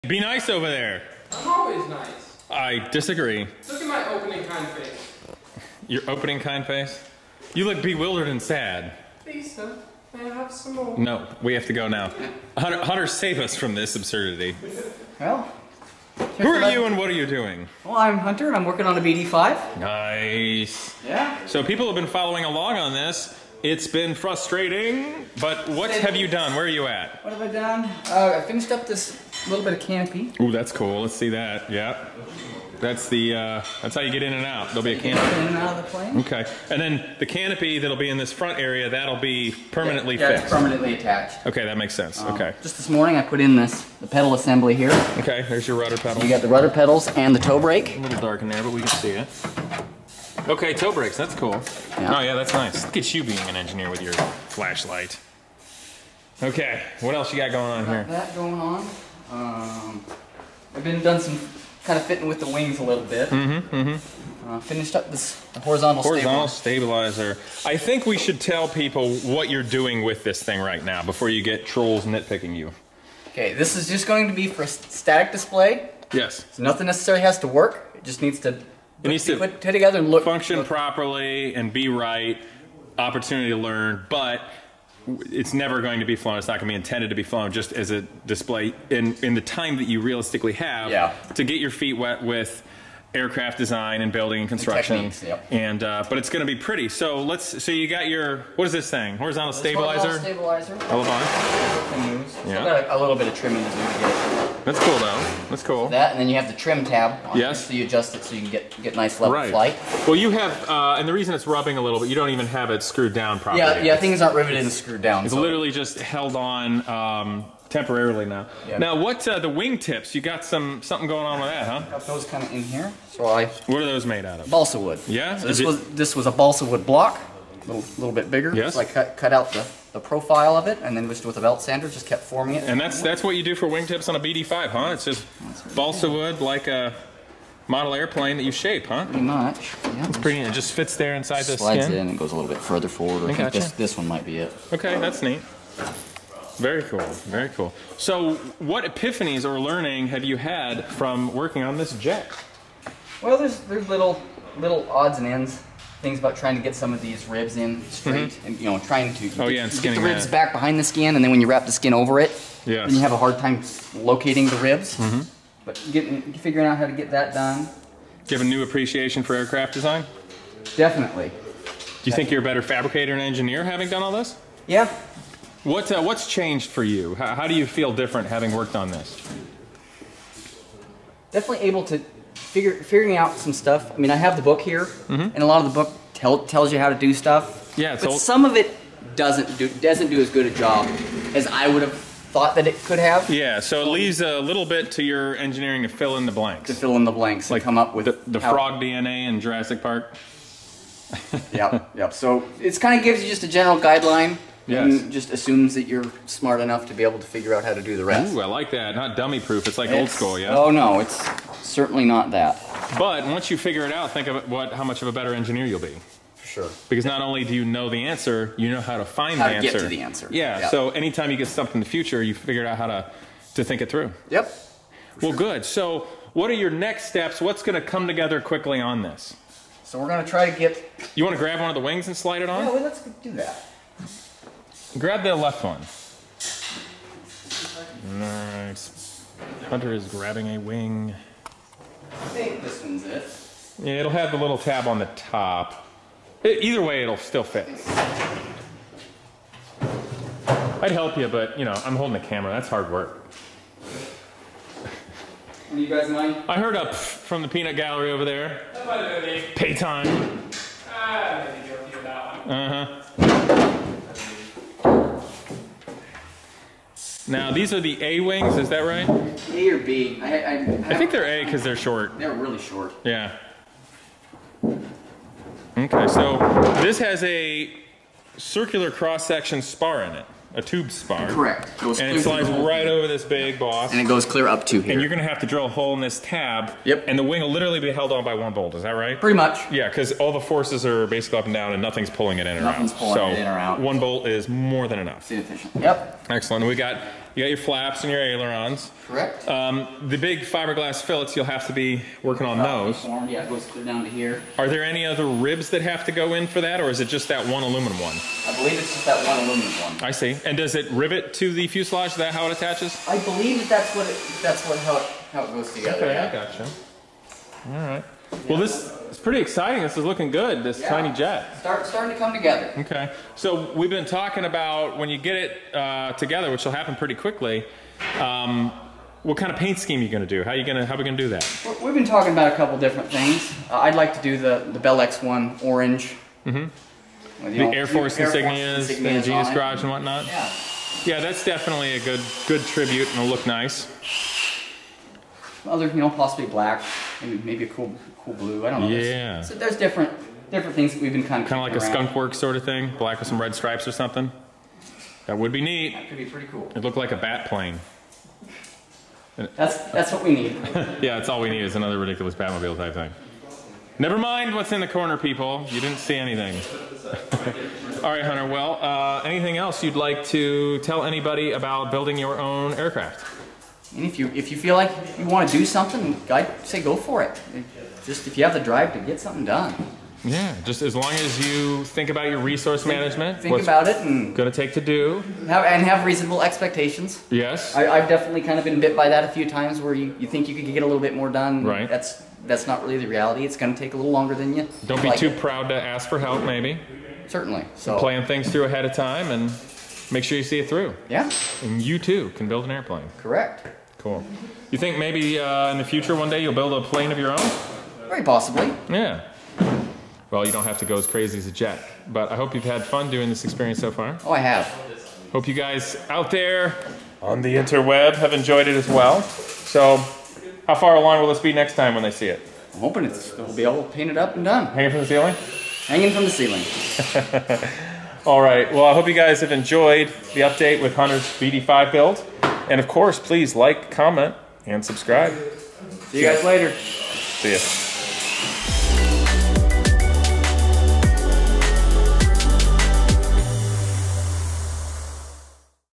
Genius Garage BD5 experimental jet build update
Genius Garage student gives an update on his BD5 micro jet build and its really getting closer to the finish line. Genius Garage